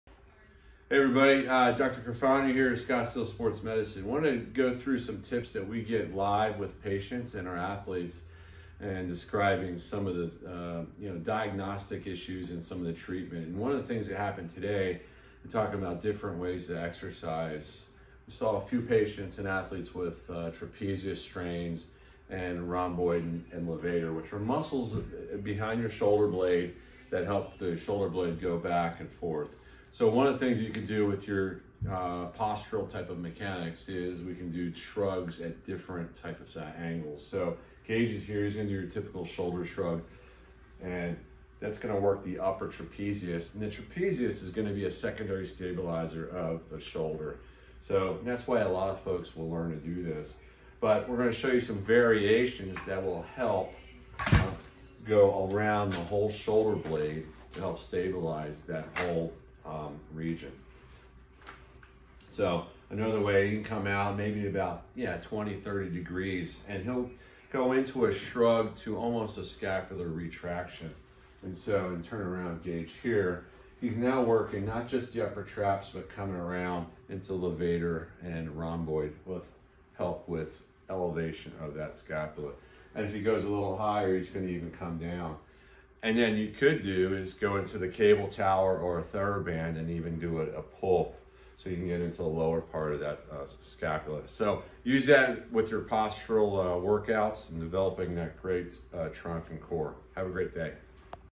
Shoulder Shrugs Variations
Traditionally Shoulder Shrugs Target the trapezius muscle group. By changing the angle of the pull of the resistance whether kettle bells, dumbbells, Therabands or a cable tower…￼￼ you can target different areas from the high to low trapezius to the levator/rhomboid region of the scapula and into the medial border of the scapula from superior to inferior.￼